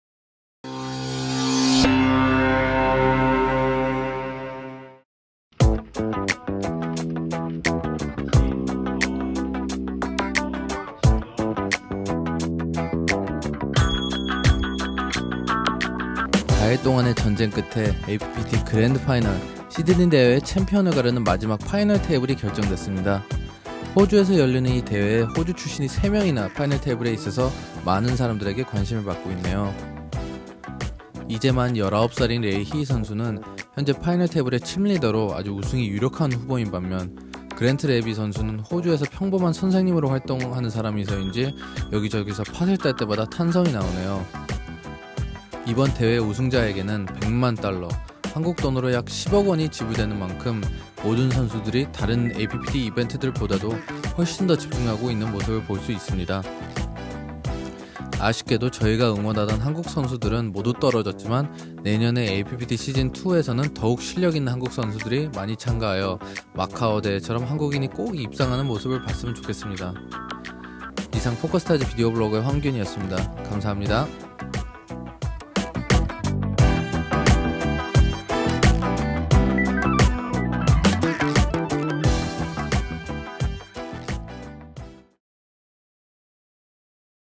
APPT Final Table Sydney (korean)
Hoon overviews events from the Final Table of the APPT Grand Final